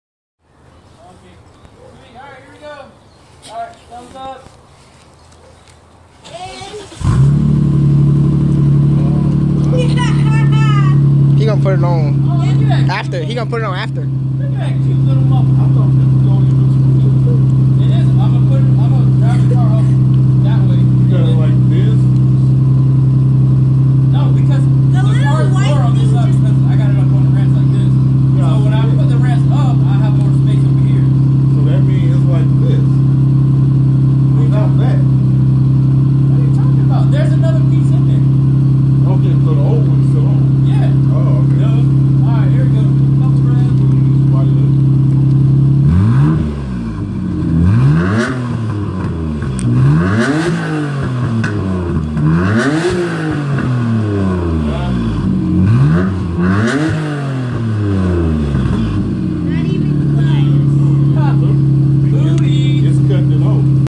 Tomei Ti with silencer R32 GTR
Here i have my little brother, cousin, and uncle helping out with recording sorry for the talking in the beginning but it sounds good overall and the neighbors are happy with it having a Silencer atleast . Now to see without...